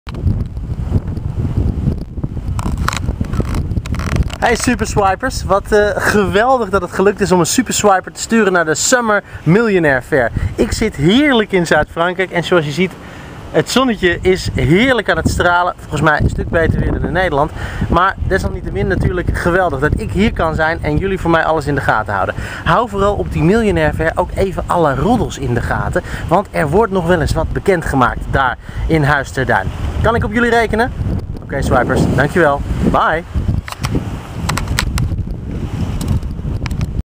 Winston Gerschtanowitz vanuit Frankrijk
Omdat Winston Gerschtanowitz op vakantie is tijdens the Miljonair Summer Fair, heeft hij een SuperSwyper ingeschakeld

Een SuperSwyper is je persoonlijke, razendsnelle journalist die je overal heen kunt sturen en van minuut tot minuut verslag doet. Dus ben je nieuwsgierig naar wat er in je afwezigheid gebeurt op een verjaardag, een vergadering of een concert waar je zelf niet bij kunt zijn? Stuur een SuperSwyper.

SuperSwypers zijn supersnel, omdat ze gebruik maken van Swype op de Samsung Galaxy S. Swype is de nieuwe invoermethode voor touchscreens, waarmee je in één aansluitende sleepbeweging over je touchscreen woorden in kan voeren en daardoor 1,5 keer sneller bent met typen.
De service is opgezet door Samsung Mobile.